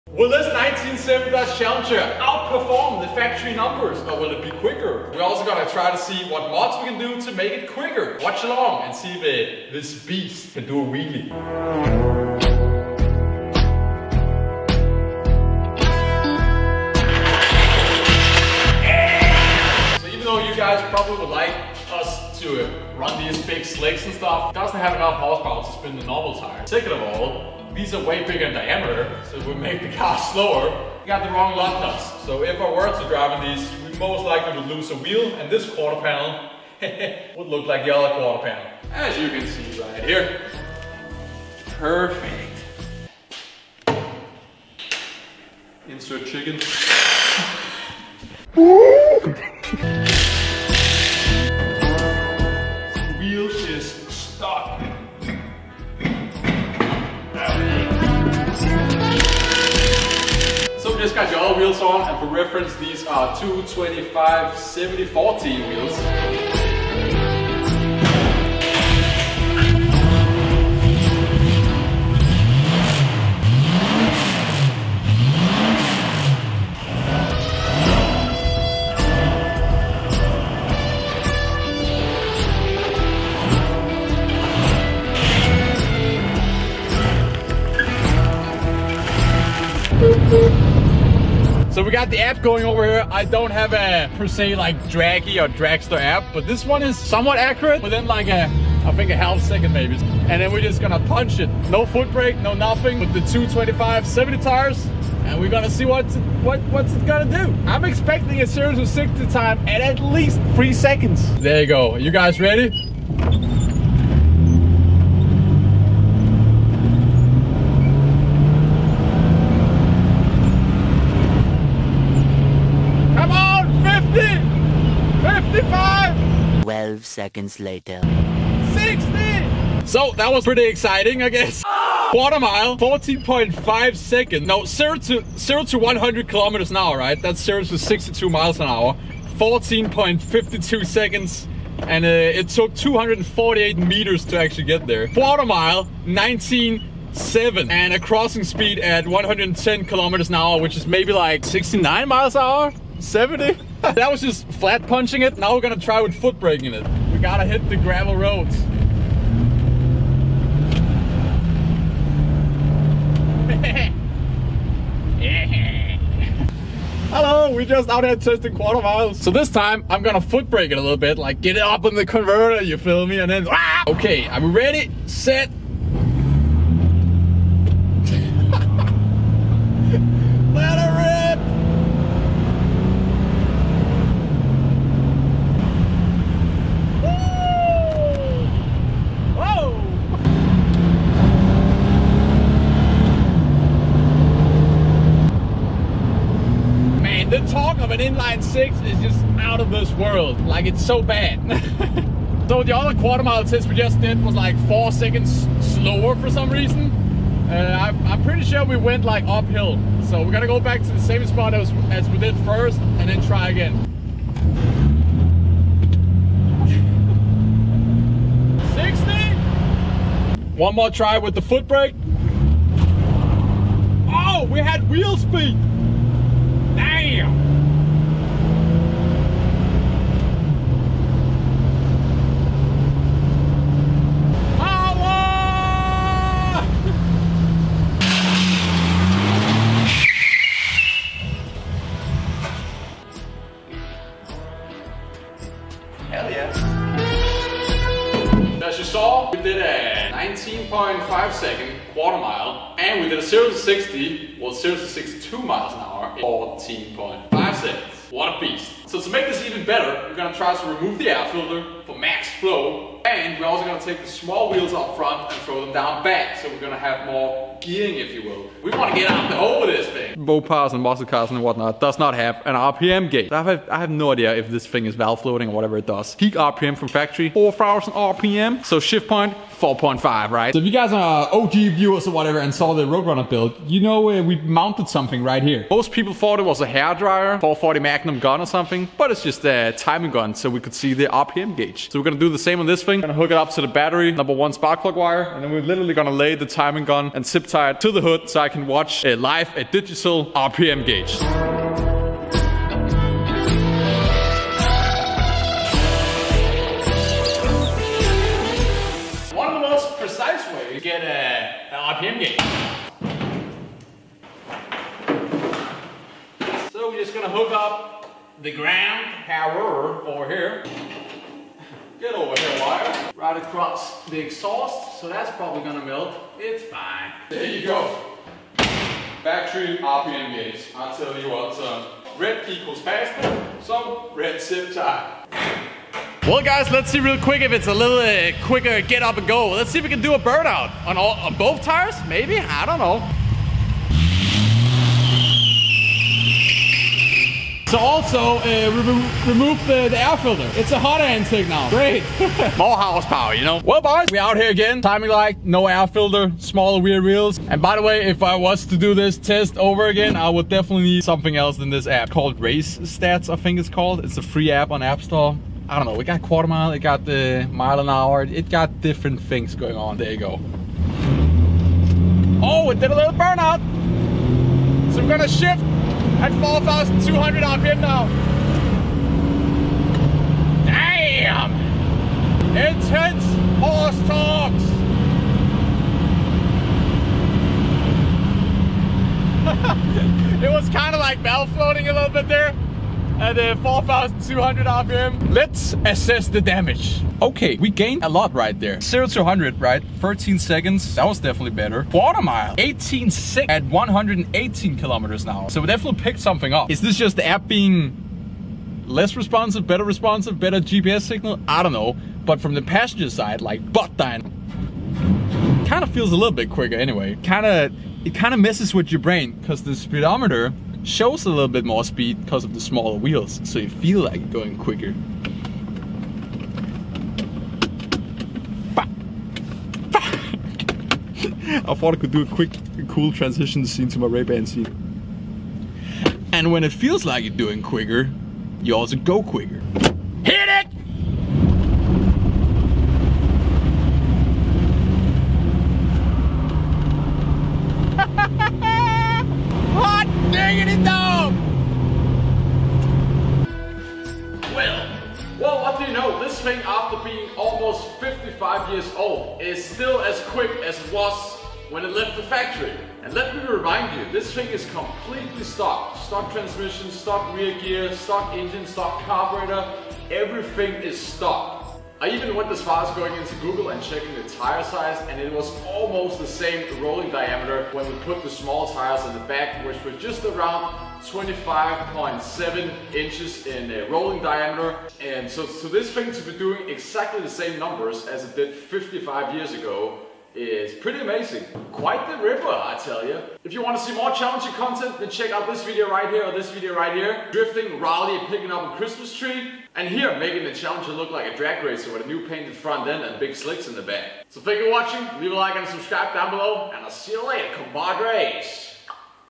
How Quick Is A Stock 1970 Dodge Challenger After 50 YEARS!
Here how fast dodge though the challenger was with the 225 Six TorqueFlite 3 speed 145 hp 0-100 km/h in 13.3 sec and a quarter mile time is 18.9 sec. which we beat 54 years later unrestored or rebuild anything, this pretty good even tho the 0 to 60 app might not be that great. who cares just a fun video!

0:00 intro
0:30 cold start
1:00 Factory Numbers
1:30 First tests
3:57 make the car quicker
5:00 Wheel swap
5:10 Rpm gauge install
5:40 Burnout Test
6:00 Final tests
6:40 Results/numbers
7:30 Faster then factory